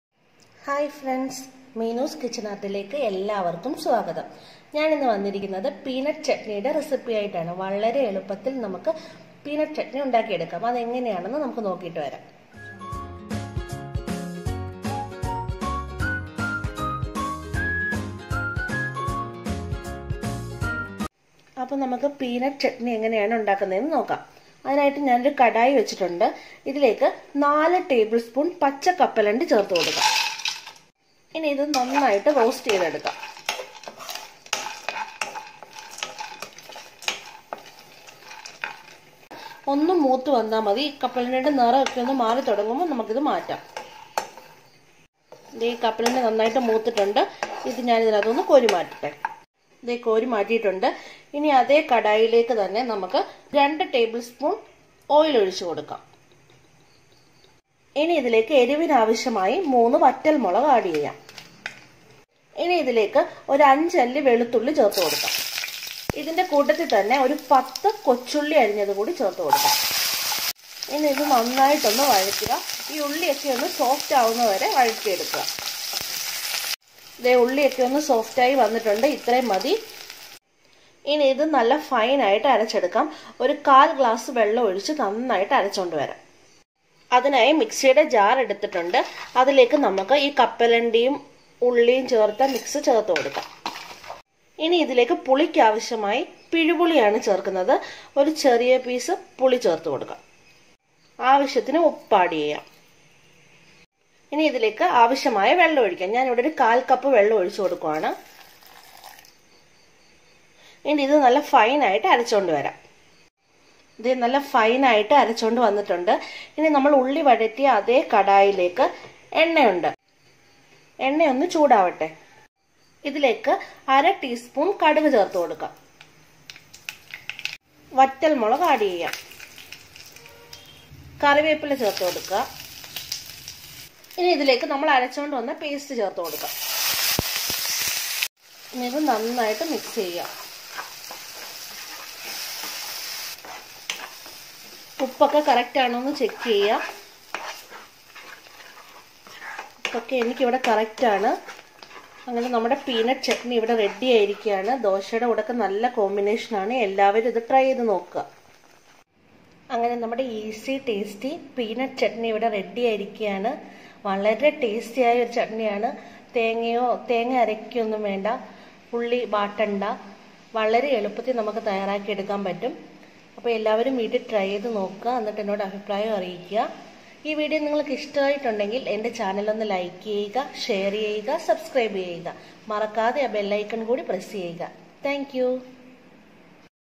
Easy Tasty Peanut Chutney || Chutney For Idly , Dosa || South Indian Special Chutney Recipes
Credits GFX Design/Channel Art/Logo/Intro Instagram: Ig_Cameronind